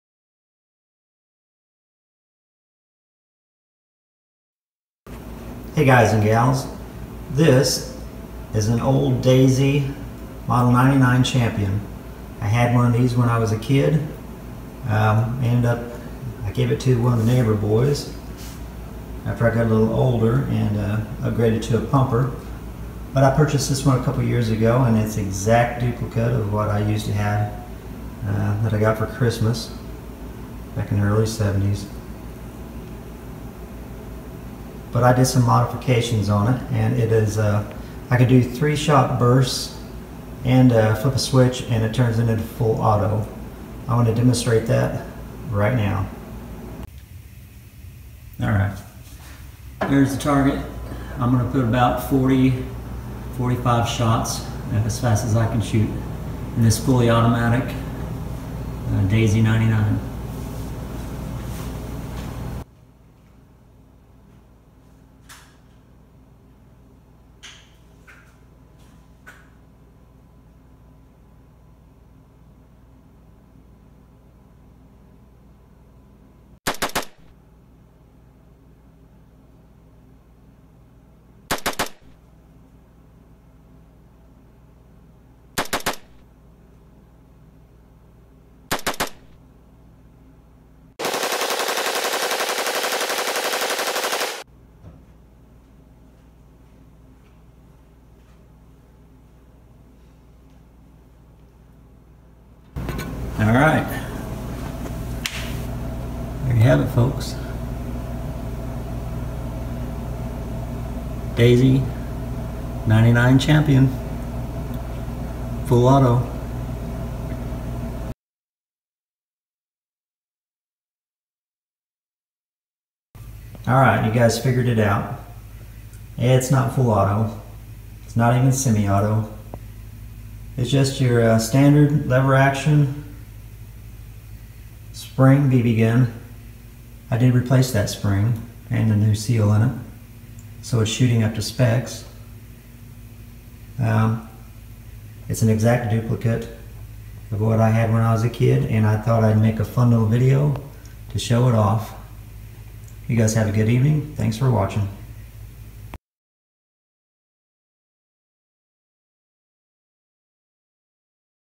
Daisy 99 Champion Full Auto
Accuracy using Full Metal Jacket Copperheads at 10 Meters